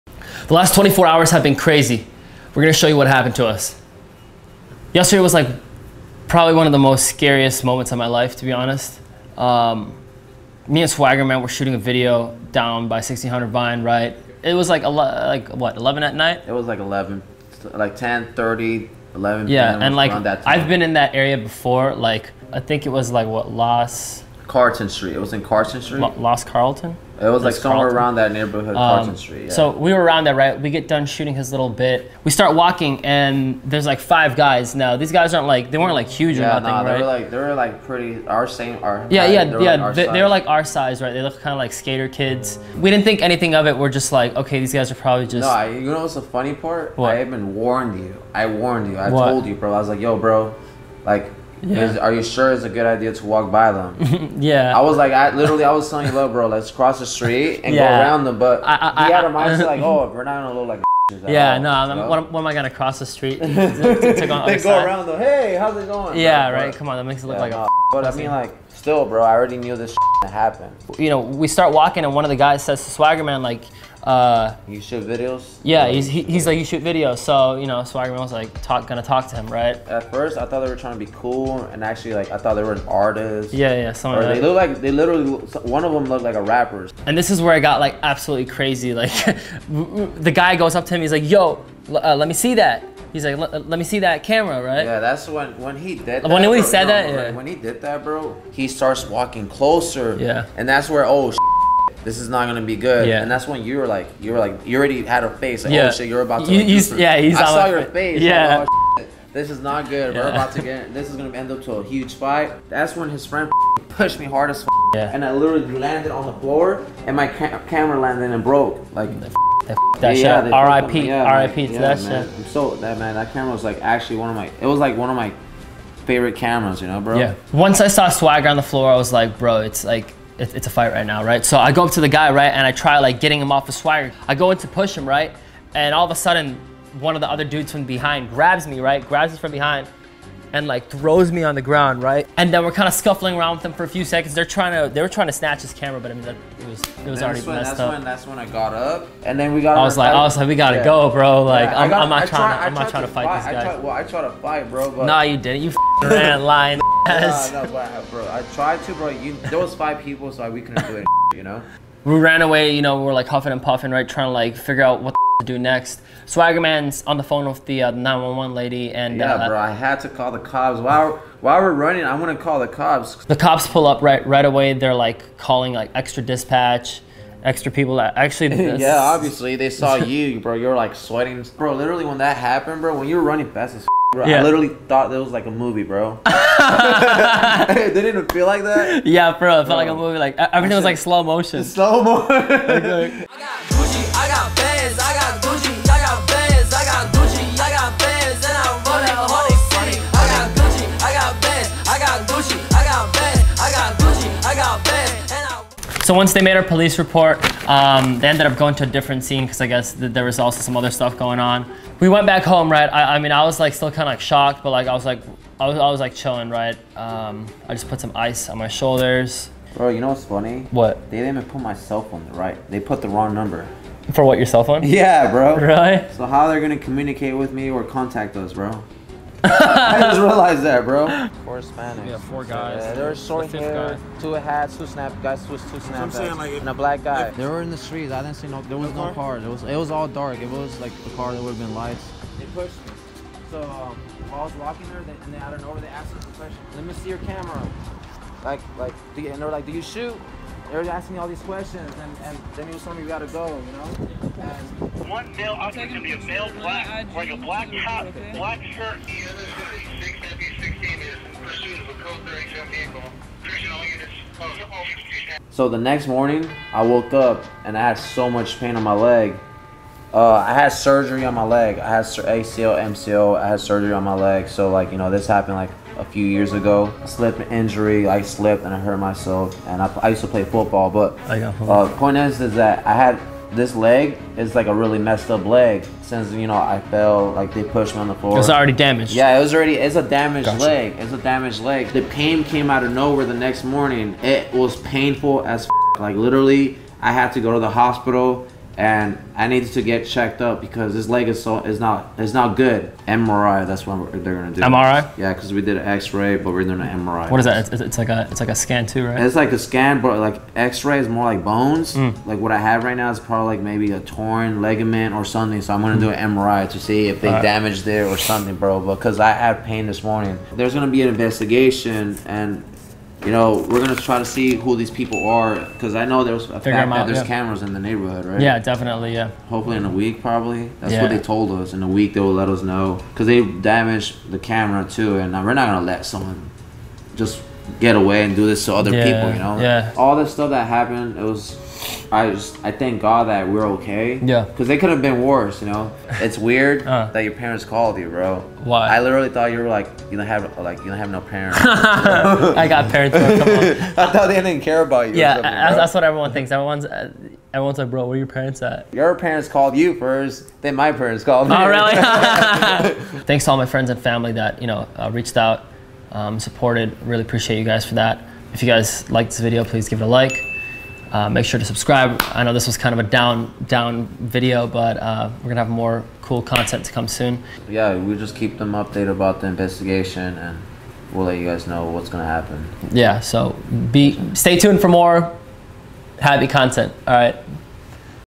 i got robbed & beat up! (not clickbait)
I usually don't make videos like this but I wanted to make sure people heard our story. Los Angeles isn't as safe as you may think. Thank God we are okay. I appreciate the support of all my friends & fans, love you guys!

Other people in the video:

Swaggy - @swagrman